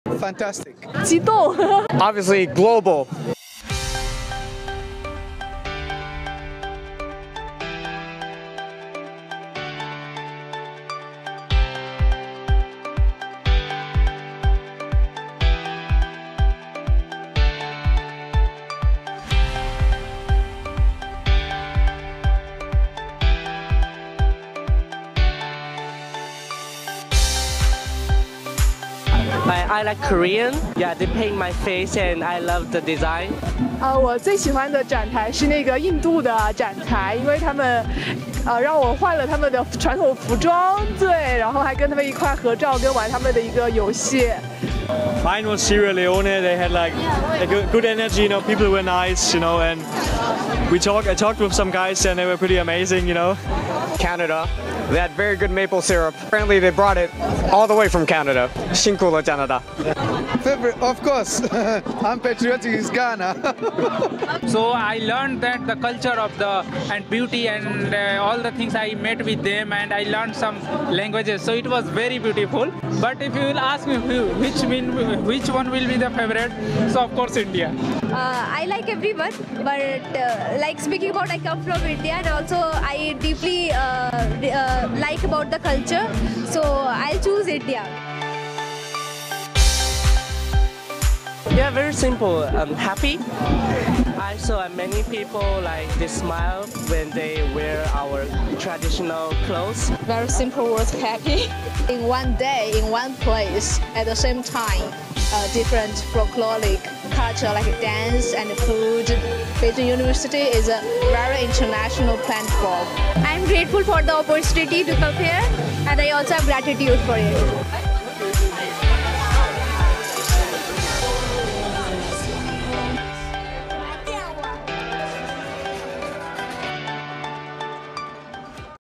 ICF2023 | Q&A at Peking University International Culture Festival On Campus Exhibition & Carnival
🎈In one day
🌐In one place
🌍Meeting the world on PKU Campus!

Tap into the excitement and join our students for a vibrant cultural journey at the PKUICF On Campus Exhibition & Carnival! 🎉

🎦: Fumika Nakahara, Tan Yee, Khoo Yeu Her, Bao Rui

WeChat: PKU_Eng